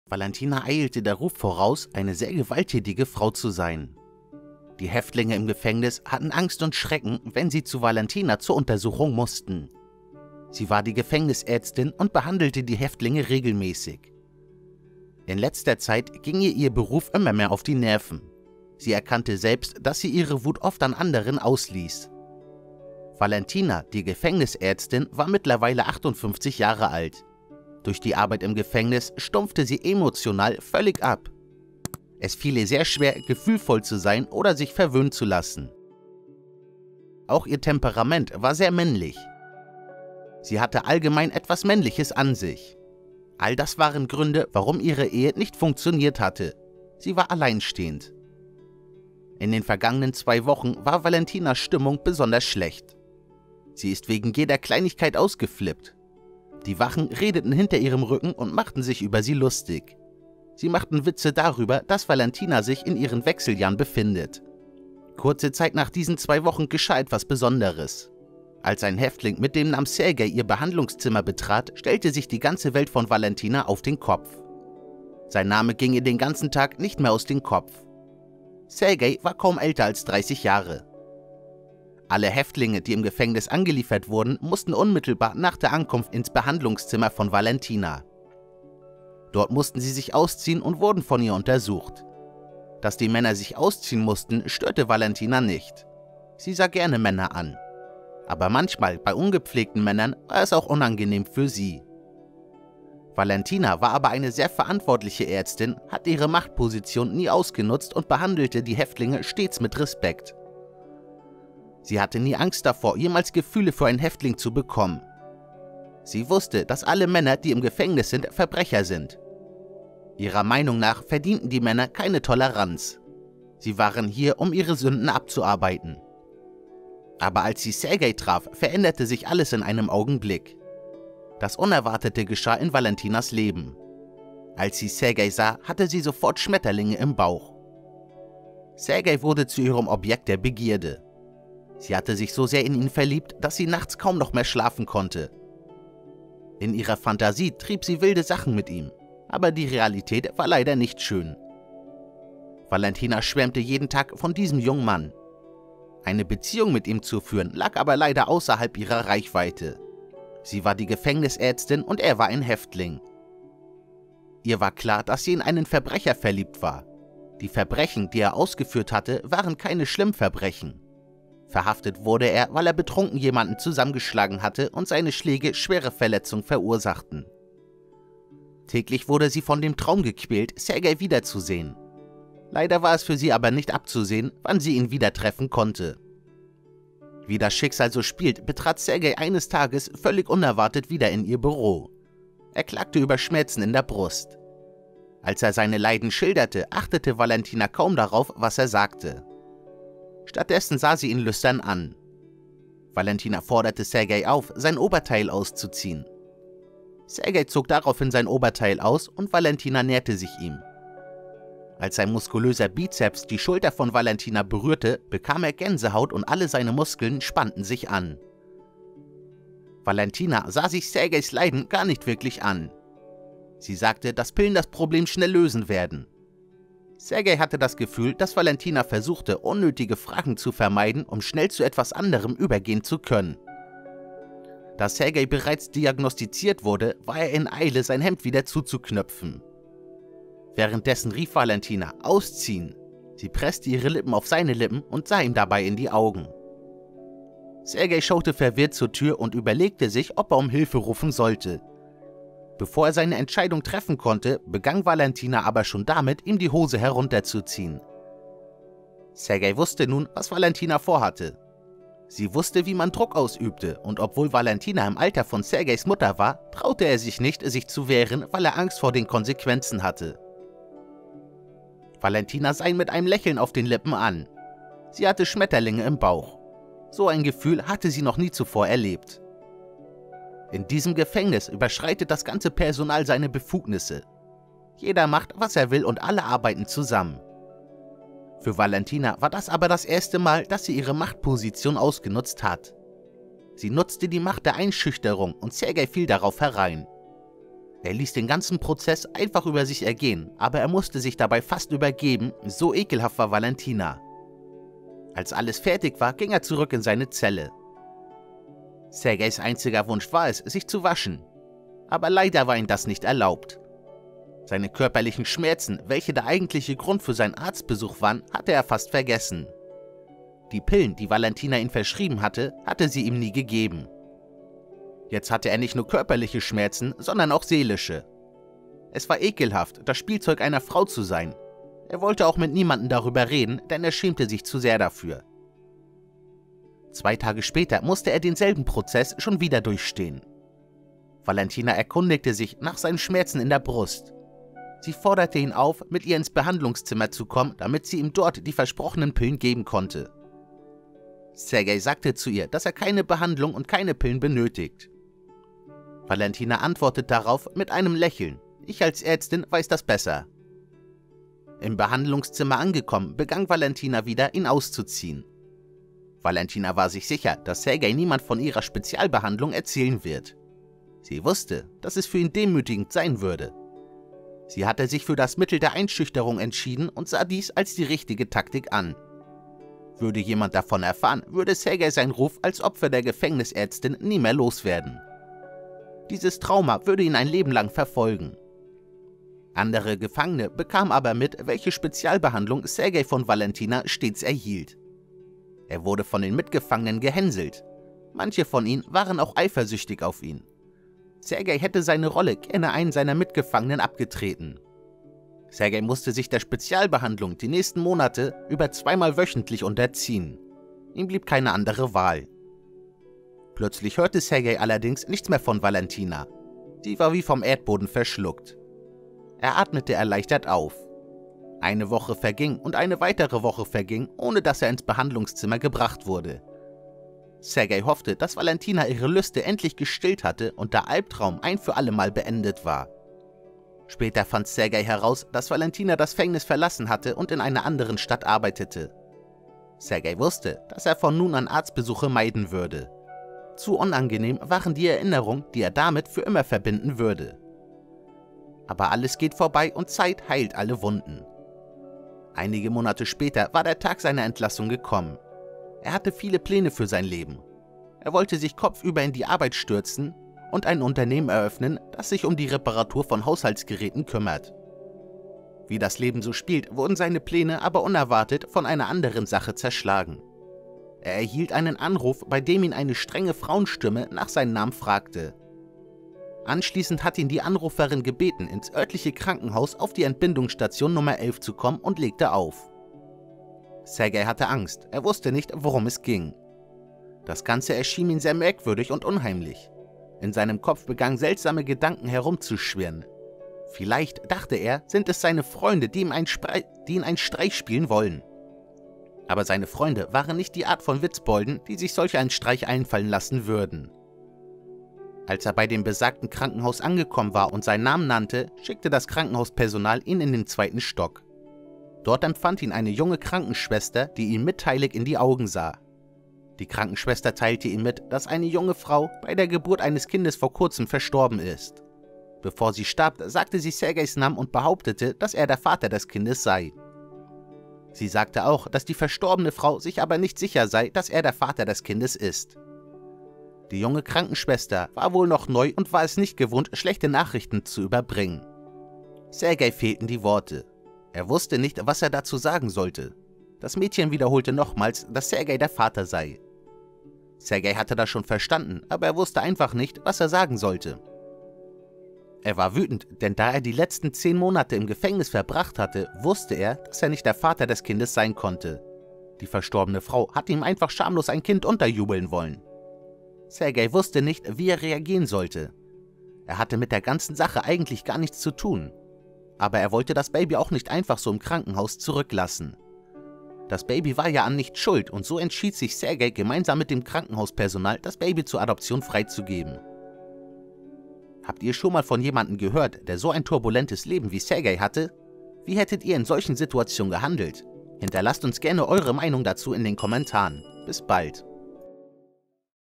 Die Gefängnisärztin brachte gutaussehenden Sträfling in ihr Büro. Lass uns ausziehen, ich hab lang..
Die Gefängnisärztin brachte einen gut aussehenden Sträfling in ihr Büro. Lass uns ausziehen, sonst habe ich es schon lange nicht mehr.

[Quellen]
Bild: videoblocks, Wikimedia, Pixabay, ...